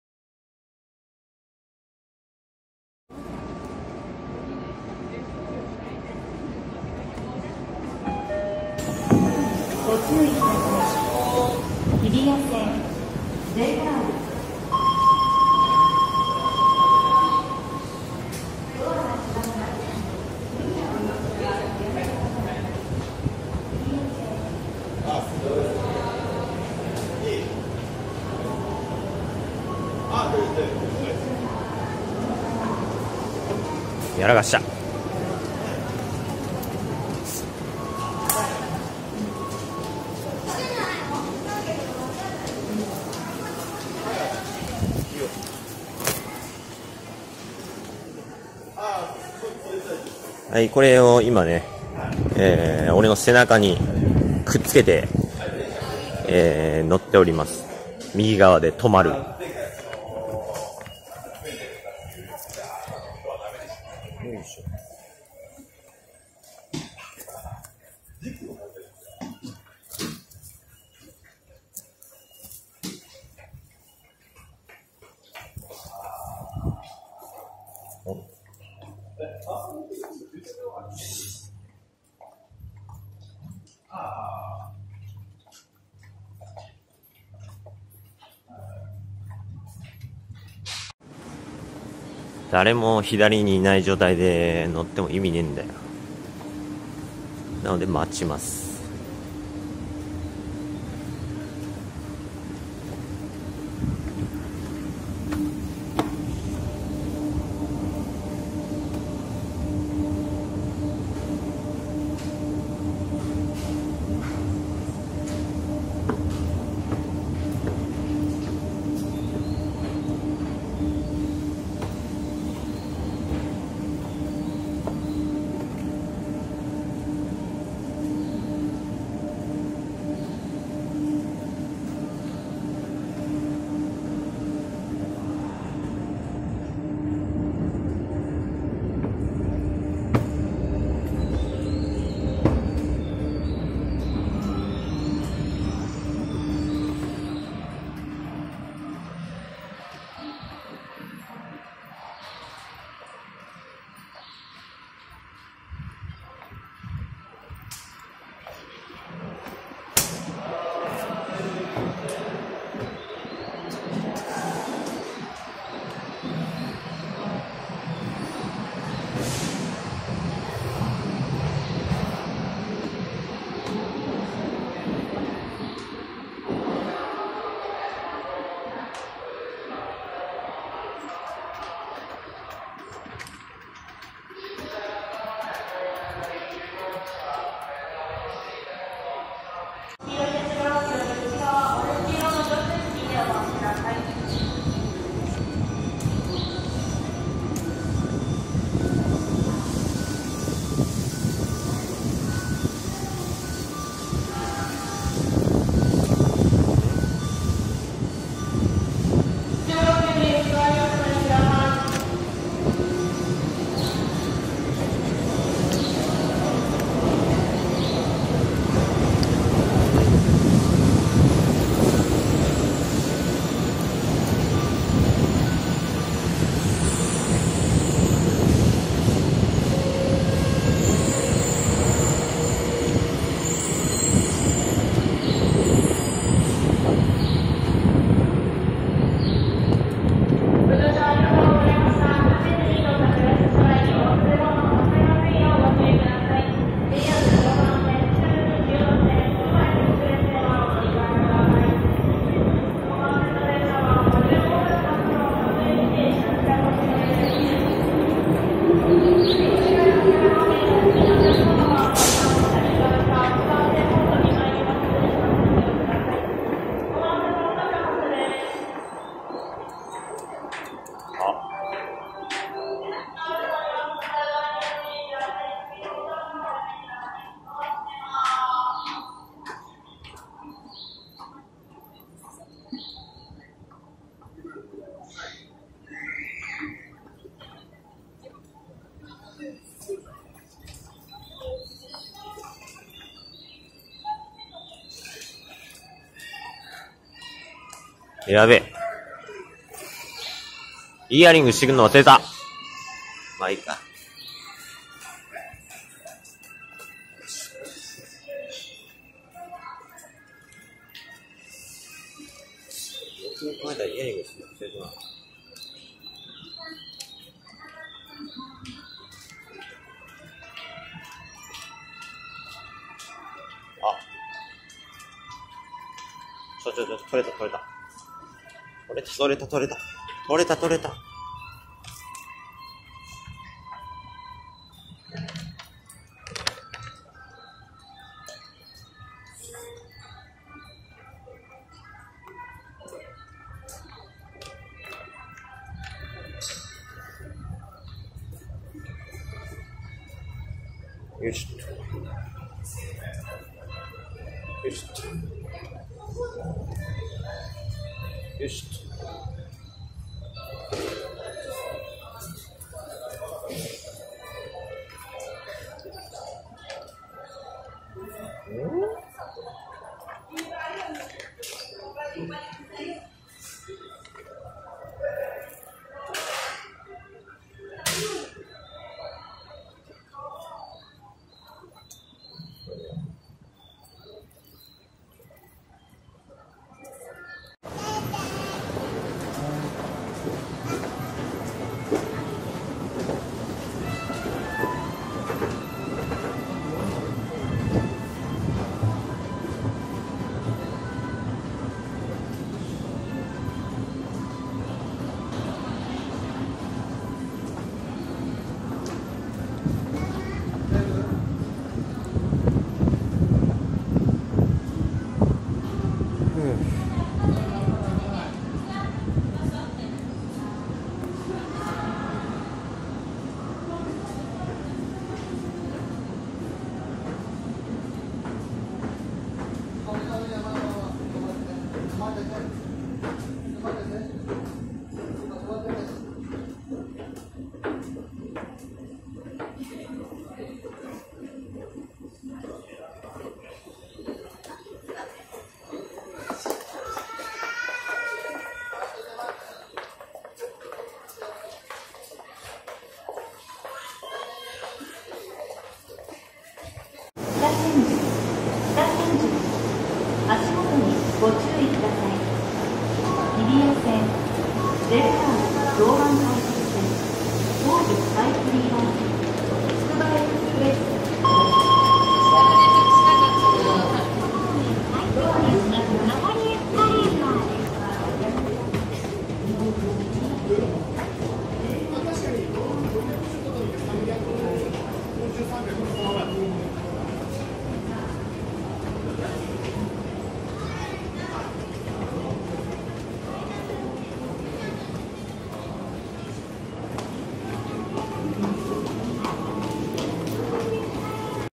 「エスカレーター、止まって乗りたい人がいる」2019年8月16日、ポスターを背中に貼って敢えて右側で止まる行為をしてみた。
この動画以降からは「エスカレーター、止まって乗りたい人がいる」が書かれているポスターを背中に貼って右側で止まります。

Twitter⇒@yukkituikyasu